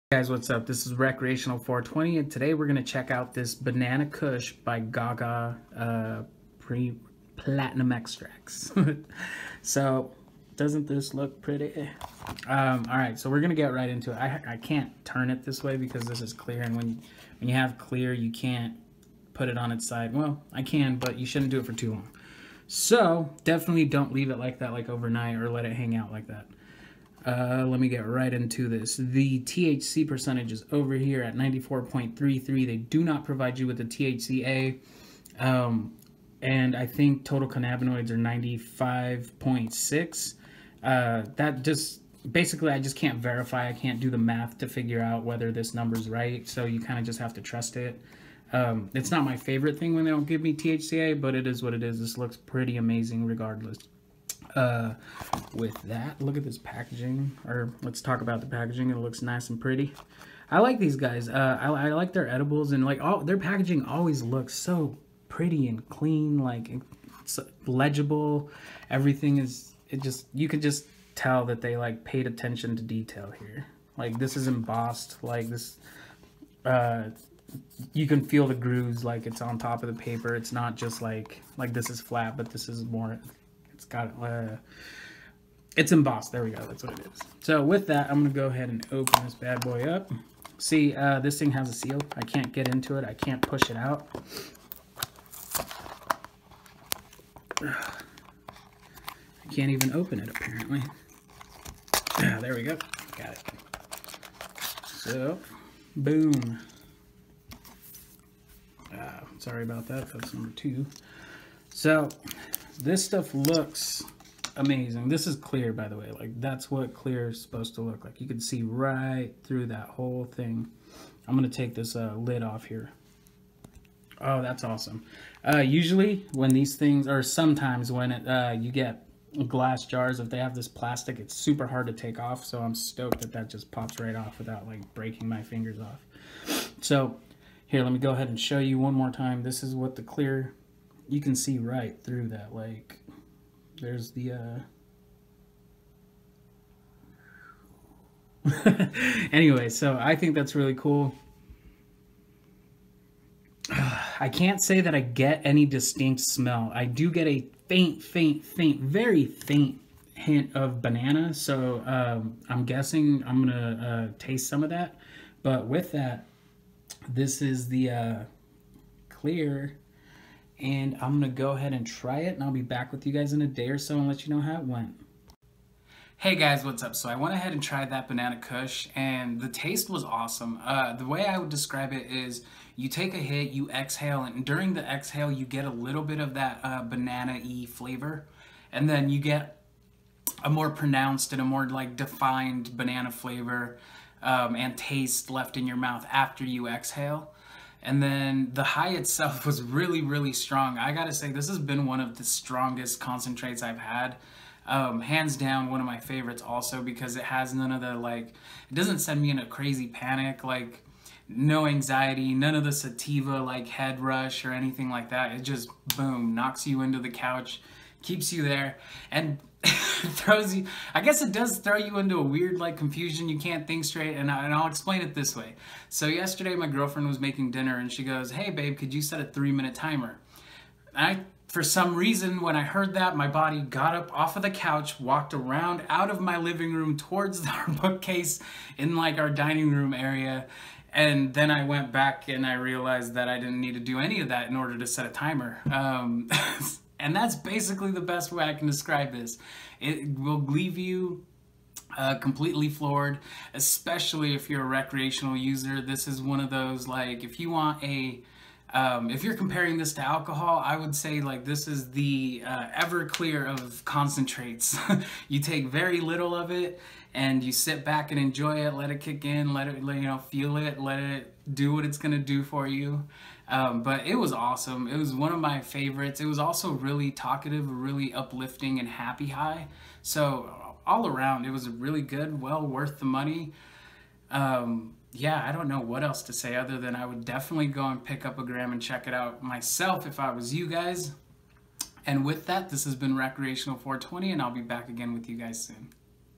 Banana Kush Clear By GaGa Platinum Extracts Recreational Cannabis Review
This is Banana Kush Clear By GaGa Platinum Extracts and lets just say this is my new favorite recreational cannabis product in Washington state right now. The taste while not being overwhelming was very much there and very pleasant. The high was a knock out. The thc % is at 94% and I don't doubt that for one second. If you happen to find them at you favorite retailer grab a gram and give it a go and if they don't I would threaten legal action until they do lol but in all seriousness do yourself a favor and try these guys out.

Thc- 94.33%

21+  recreational cannabis review. Not employed by any Grower, Processor or retailer.